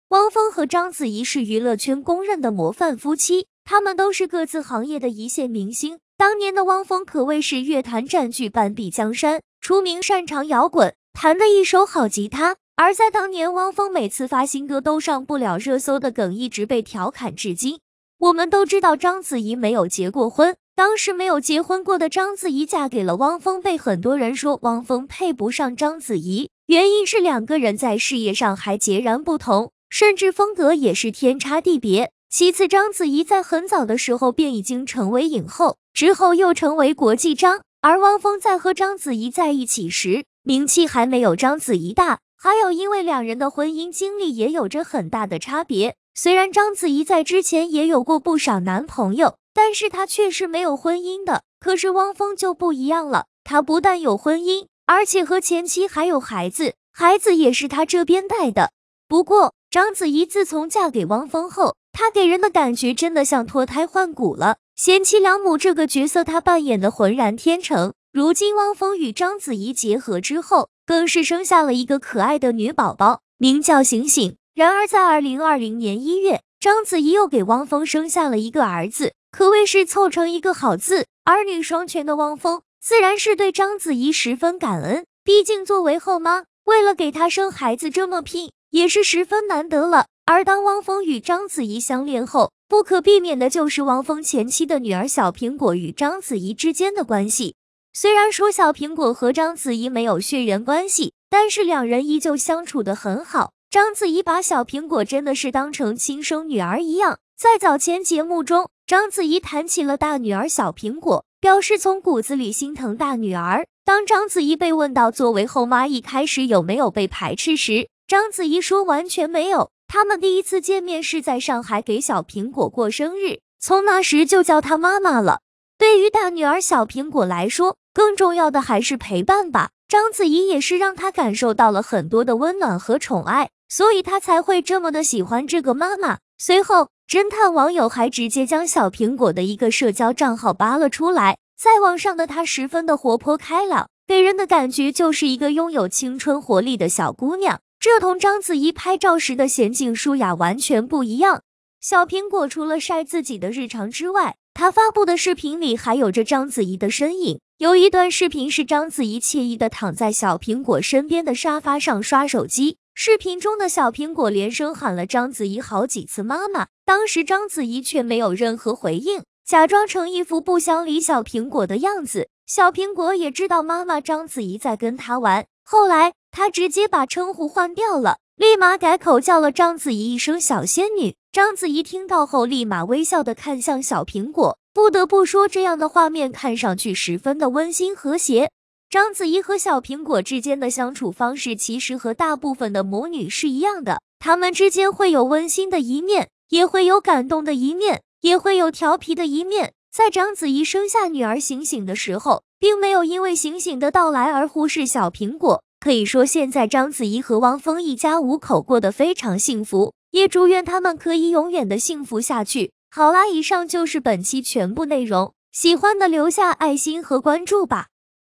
汪峰前妻女儿叫章子怡妈妈，章子怡都不理，下秒女儿的反应绝了！
汪峰和章子怡是娱乐圈公认的模范夫妻，他们都是各自行业的一线明星。当年的汪峰可谓是乐坛占据半壁江山，出名擅长摇滚，弹得一手好吉他，而在当年汪峰每次发新歌都上不了热搜的梗一直被调侃至今。
我们都知道章子怡没有结过婚，当时没有结婚过的章子怡嫁给了汪峰被很多人说汪峰配不上章子怡，原因是两个人在事业上还截然不同，甚至风格也是天差地别，其次章子怡在很早的时候便已经成为影后，之后又成为国际章，而汪峰在和章子怡在一起时，名气还没有章子怡大。还有因为两人的婚姻经历也有着很大的差别，虽然章子怡在之前也有过不少男朋友，但是她却是没有婚姻的，可是汪峰就不一样了，他不但有婚姻，而且和前妻还有孩子，孩子也是他这边带的。
不过，章子怡自从嫁给汪峰后，她给人的感觉真的像脱胎换骨了，贤妻良母这个角色她扮演得浑然天成。如今汪峰与章子怡结合之后，更是生下了一个可爱的女宝宝，名叫醒醒。然而在2020年1月，章子怡又给汪峰生下了一个儿子，可谓是凑成一个“好”字。儿女双全的汪峰，自然是对章子怡十分感恩，毕竟作为后妈，为了给他生孩子这么拼，也是十分难得了。而当汪峰与章子怡相恋后，不可避免的就是汪峰前妻的女儿小苹果与章子怡之间的关系。
虽然说小苹果和章子怡没有血缘关系，但是两人依旧相处的很好。章子怡把小苹果真的是当成亲生女儿一样。在早前节目中，章子怡谈起了大女儿小苹果，表示从骨子里心疼大女儿，当章子怡被问到作为后妈一开始有没有被排斥时，章子怡说完全没有，她们第一次见面是在上海给小苹果过生日，从那时就叫她妈妈了。
对于大女儿小苹果来说，更重要的还是陪伴吧。章子怡也是让她感受到了很多的温暖和宠爱，所以她才会这么的喜欢这个妈妈。随后，“侦探”网友还直接将小苹果的一个社交账号扒了出来，在网上的她十分的活泼开朗，给人的感觉就是一个拥有青春活力的小姑娘。这同章子怡拍照时的娴静淑雅完全不一样。
小苹果除了晒自己的日常之外，她发布的视频里还有着章子怡的身影，有一段视频是章子怡惬意地躺在小苹果身边的沙发上刷手机，视频中的小苹果连声喊了章子怡好几次妈妈，当时章子怡却没有任何回应，假装成一幅不想理小苹果的样子，小苹果也知道妈妈章子怡在跟她玩，后来，她直接把称呼换掉了，立马改口叫了章子怡一声小仙女，章子怡听到后立马微笑地看向小苹果，不得不说这样的画面看上去十分的温馨和谐。
章子怡和小苹果之间的相处方式其实和大部分的母女是一样的，她们之间会有温馨的一面，也会有感动的一面，也会有调皮的一面。在章子怡生下女儿醒醒的时候，并没有因为醒醒的到来而忽视小苹果，可以说现在章子怡和汪峰一家五口过得非常幸福，也祝愿他们可以永远的幸福下去。好啦以上就是本期全部内容，喜欢的留下爱心和关注吧！